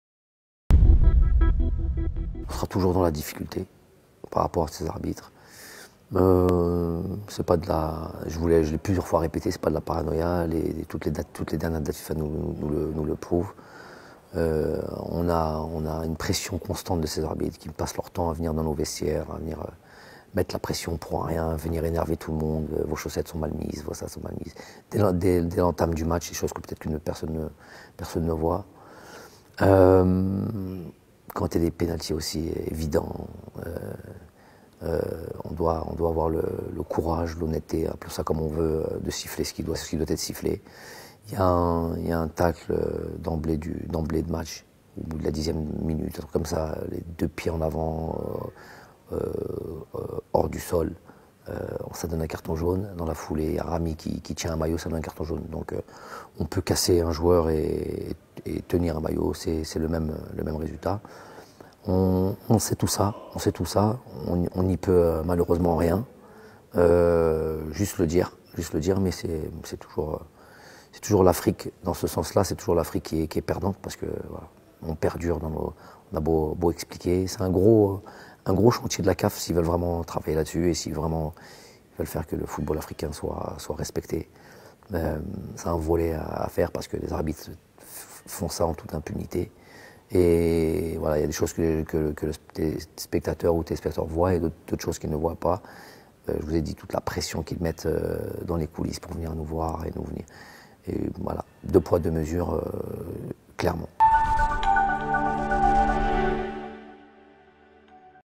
بلماضي : معاناتنا مع التحكيم في الملاعب الافريقية متواصلة
الجزائر (وأج)- اكد الناخب الوطني جمال بلماضي ، ان معاناة فريقه مع التحكيم في الملاعب الافريقية لازالت متواصلة، مشيرا خلال ندوة صحفية عقدها عقب مواجهة بوركينافاسو في اطار تصفيات كاس العالم، ان المُشكلة دائما تتكرر مع الفريق الجزائري، و الذي يتعرض في كل مرة لتجاوزات من طرف الحُكام.